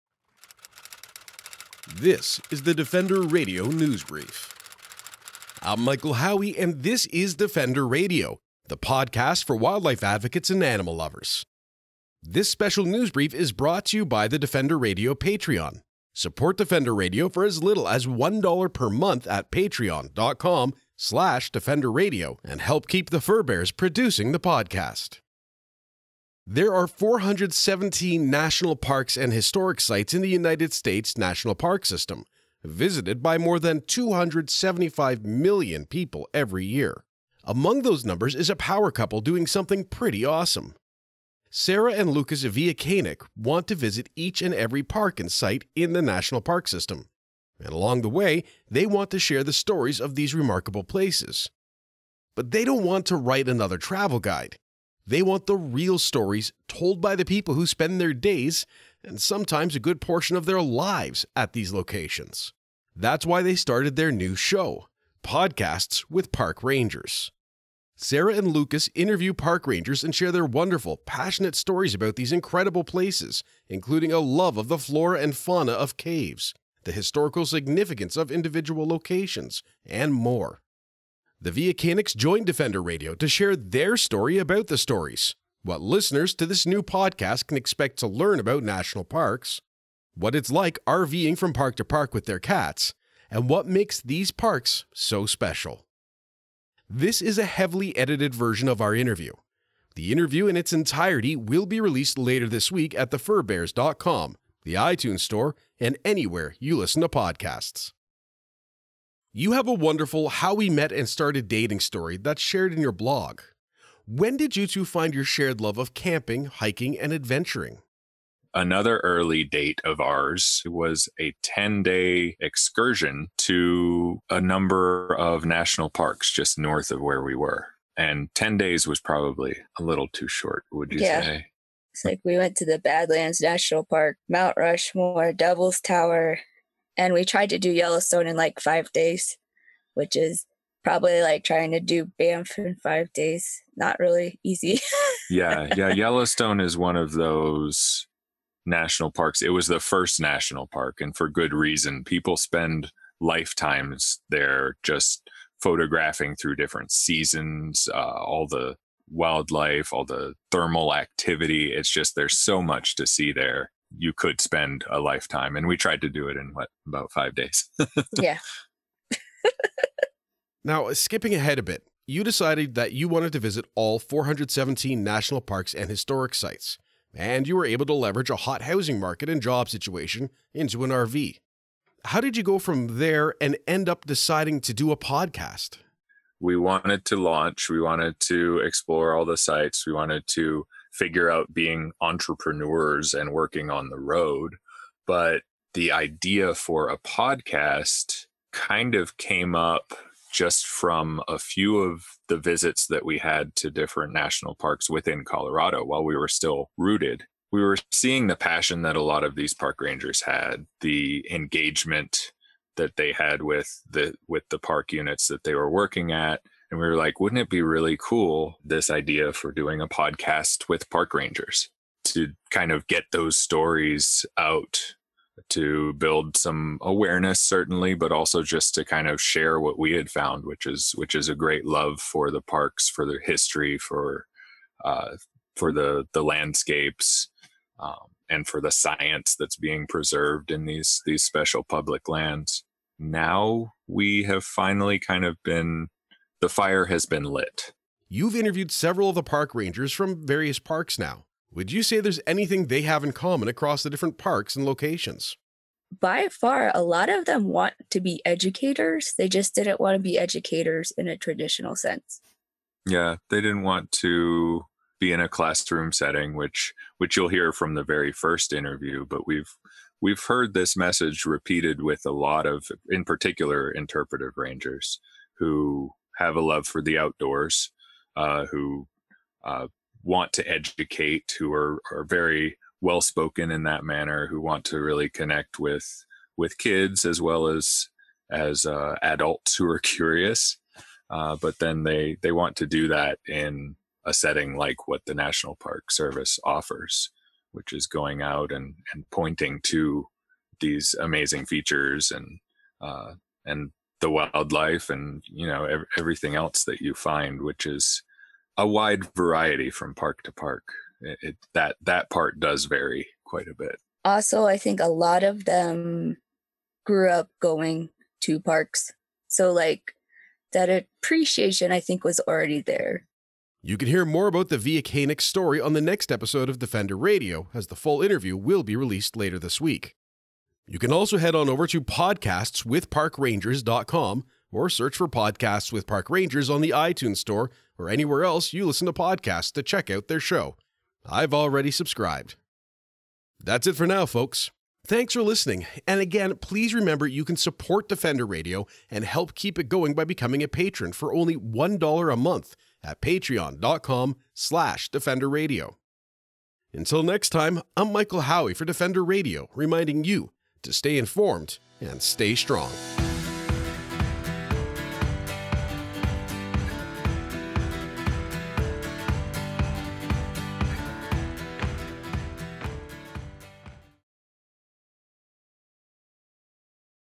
NEWS BRIEF - 525: Podcasts With Park Rangers
Sarah and Lucas Villa-Kanic want to visit each and every park and site in the national park system – and along the way, they want to share the stories of these remarkable places. But they don’t want to write another travel guide, they want the real stories, told by the people who spend their days – and sometimes a good portion of their lives – at these locations. That’s why they started their new show: Podcasts with Park Rangers.

 Sarah and Lucas interview park rangers and share their wonderful, passionate stories about these incredible places, including a love of the flora and fauna of caves, the historical significance of locations, and more. The Villa-Kanics joined Defender Radio to share their story about the stories – and what listeners to this new podcast can expect to learn about national parks, RV'ing from park to park with their cats, and what makes these locations so special.

 &nbsp;